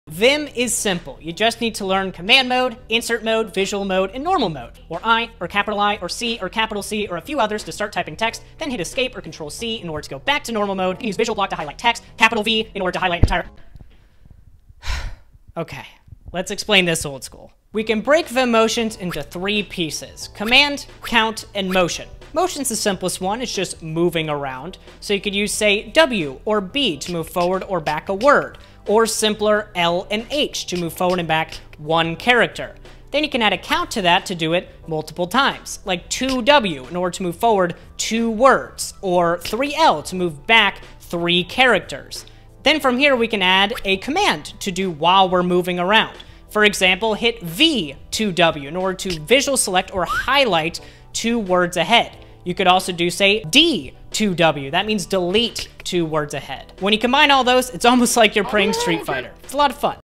Be a vim PRO in 1 minute
vim motions actually are simple. Let's break down command - count - motion to learn every combo vim has to offer.